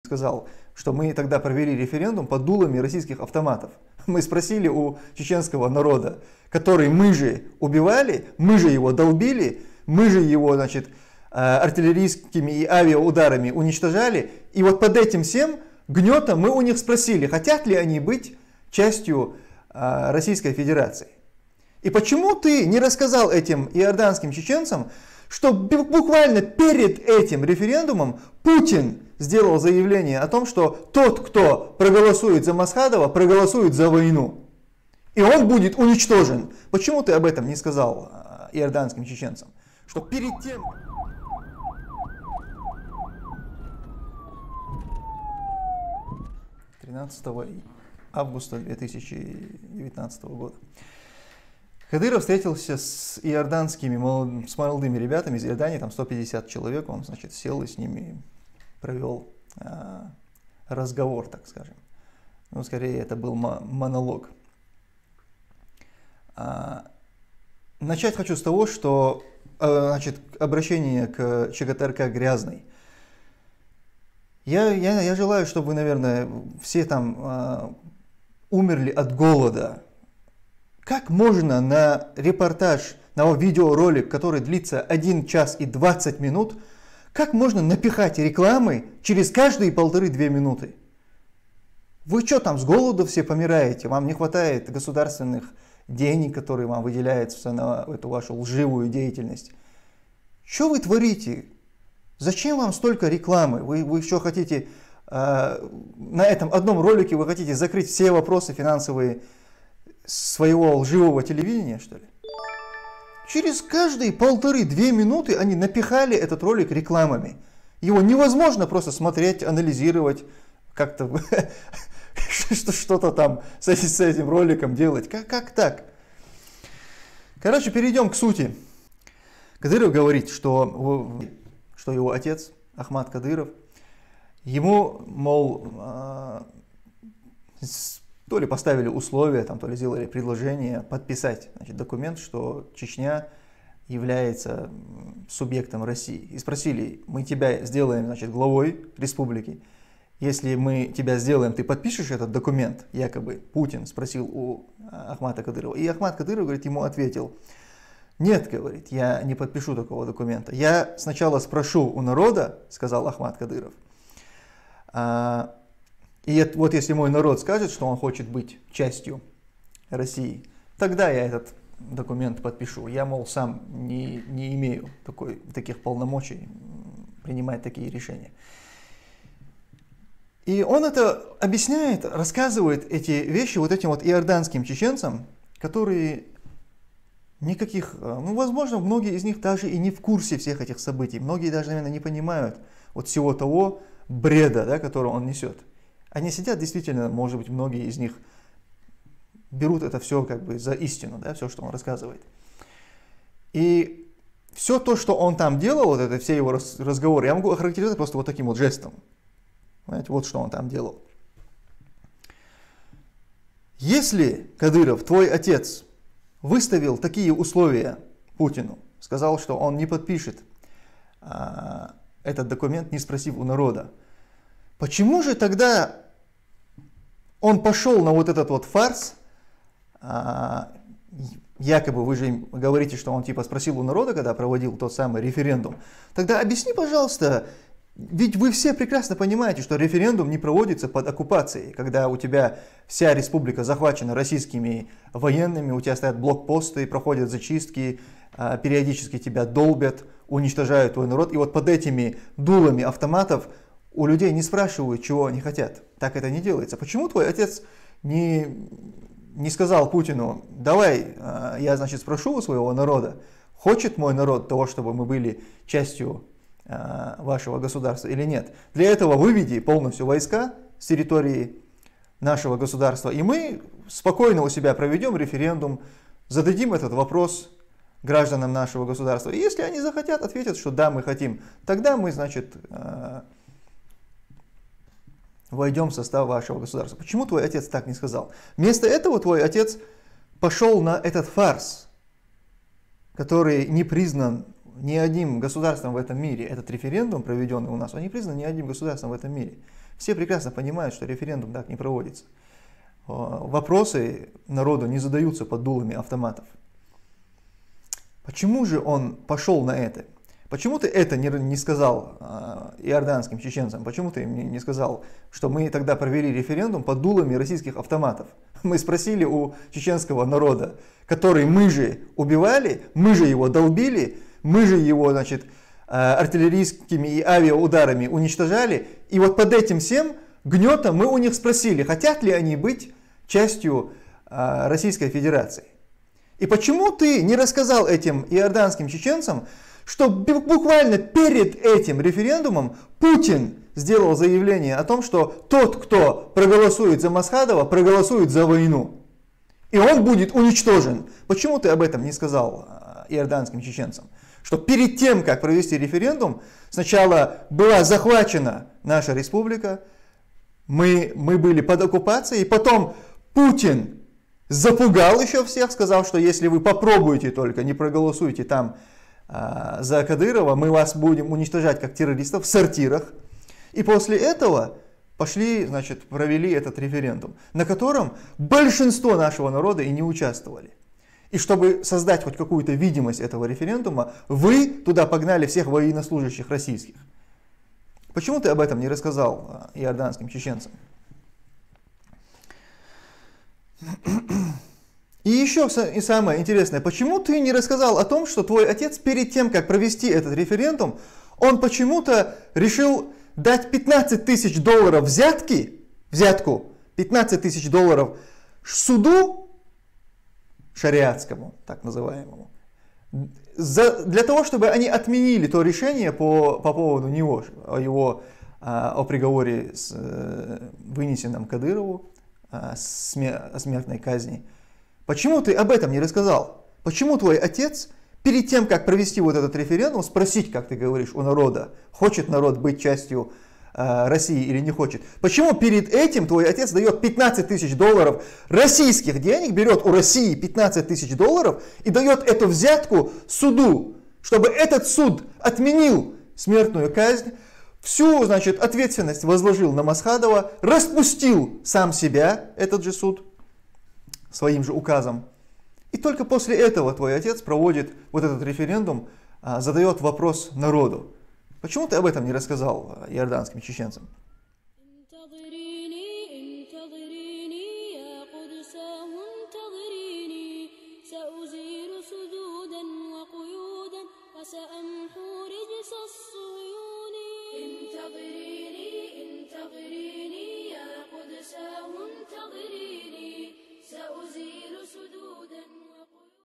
РЕФЕРЕНДУМ ПОД ОККУПАЦИЕЙ. КАДЫРОВУ ОТМЕНИЛИ РЕШЕНИЕ СУДА ВСПОМНИМ 2019Г
ВСПОМНИМ 2019Г
РЕФЕРЕНДУМ ПОД ОККУПАЦИЕЙ.
КАДЫРОВУ ОТМЕНИЛИ РЕШЕНИЕ СУДА ЗА 15000$
АЛЧНОСТЬ ЧГТРК.